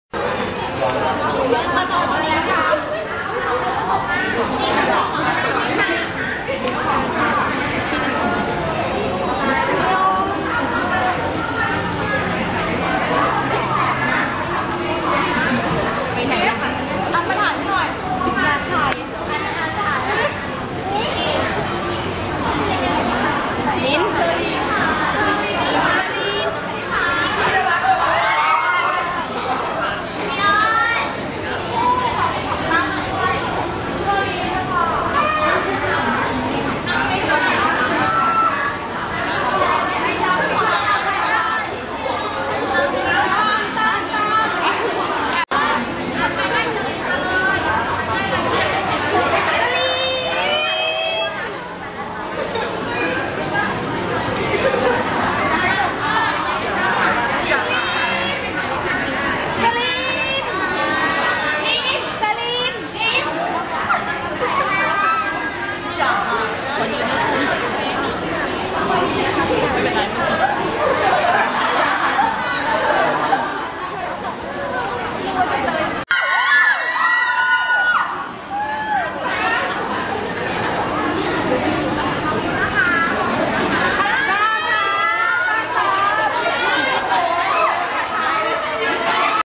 [FANCAM] อ้น เชอรีน ตั้ม รับของจากแฟนคลับ 07062013
หลังจบรายการ I AM SIAM ที่ Channel [V] Thailand น้องอ้น น้องเชอรีนและน้องตั้ม ได้ออกมารับของจากแฟนคลับค่ะ (ARพาออกมาแค่3คนน๊า ไม่ใช่ไม่ถ่ายน้องคนอื่นนะคะ^^) 07062013